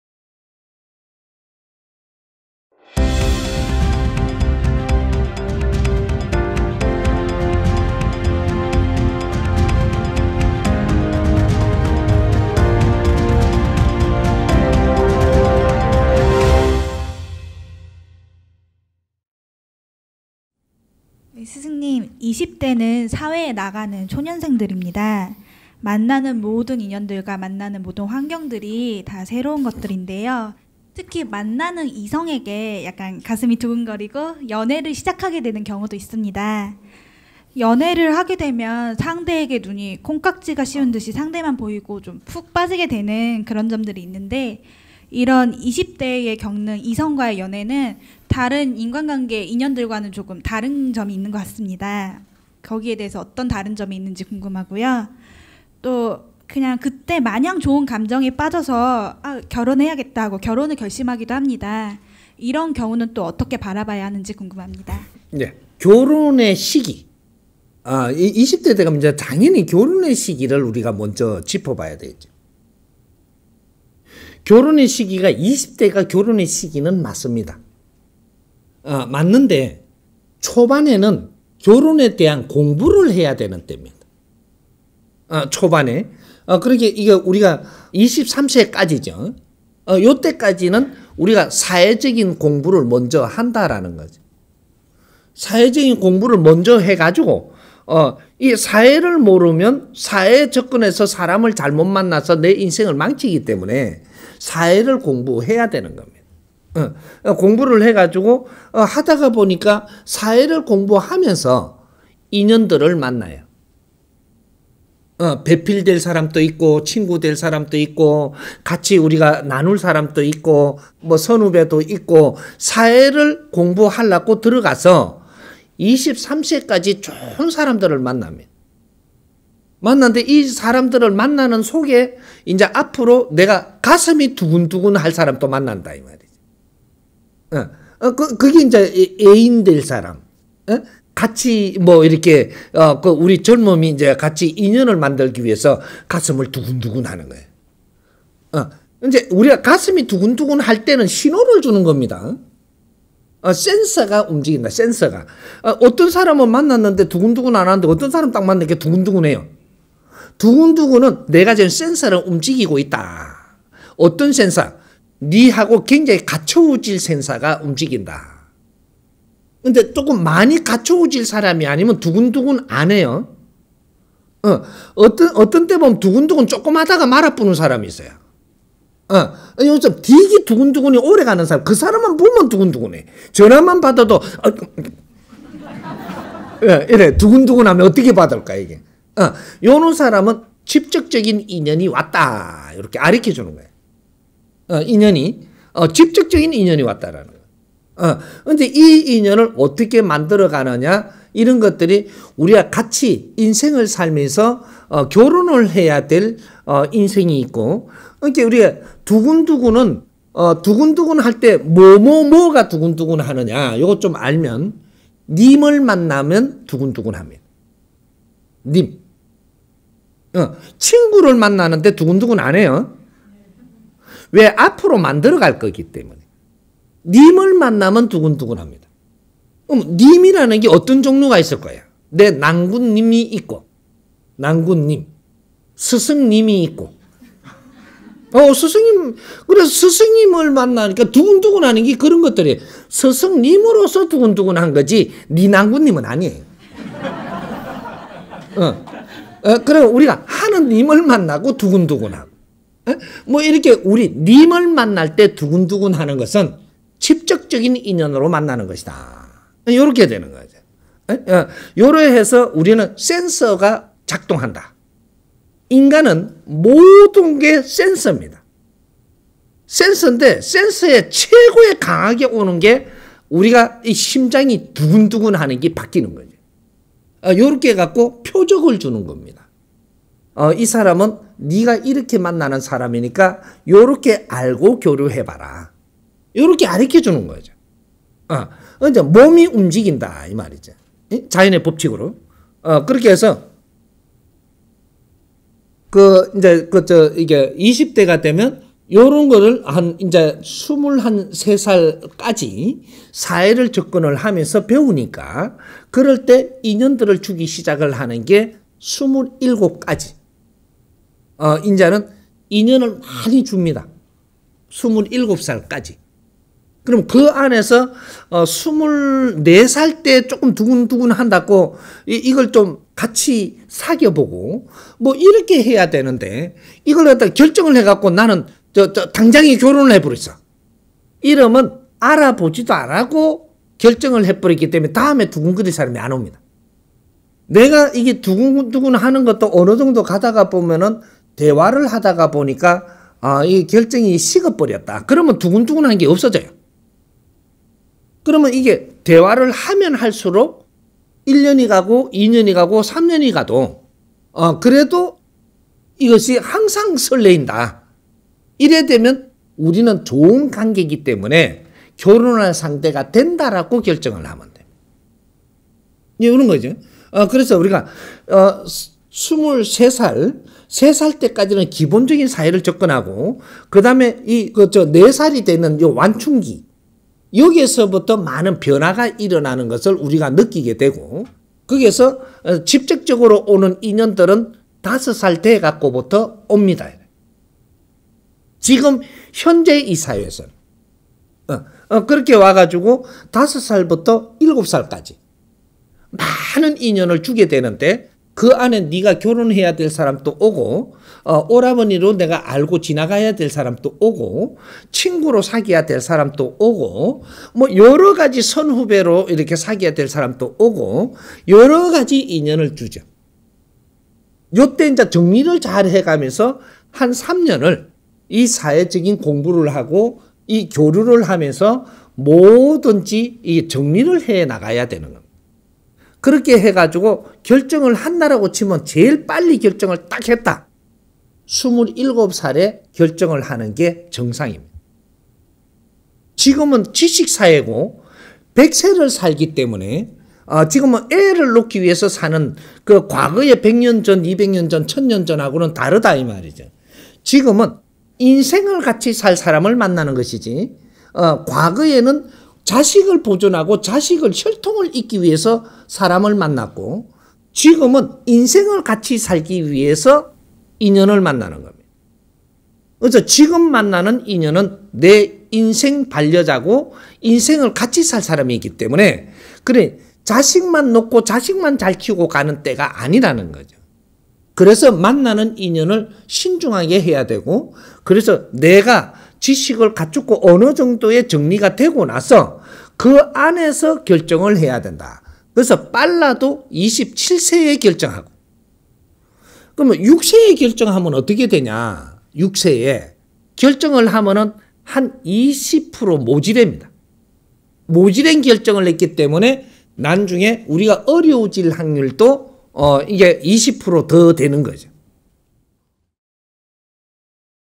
[대한민국 비메이커 포럼] 8034강 20대의 인연 그리고 연애(1_2)
질문 : 스승님 20대는 사회로 나가는 초년생들입니다. 만나는 모든 인연과 환경들이 다 새로운 것들입니다. 특히 만나는 이성에게 가슴이 두근거리고 연애를 시작하게 되는 경우도 있습니다. 연애를 하게 되면 콩깍지가 씐 듯이 상대만 보이게 되고 푹 빠지게 되는 부분들이 있는데 20대에 겪는 이성과의 연애는 다른 인간관계와 다른 점이 있는 것 같습니다. 어떤 부분이 다른지 궁금하고 또 마냥 좋은 감정에 빠져서 결혼을 결심하기도 합니다. 이런 경우는 어떻게 바라보아야 하는지 궁금합니다.

강의 일자 : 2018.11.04 서울 코엑스 / 정법시대 : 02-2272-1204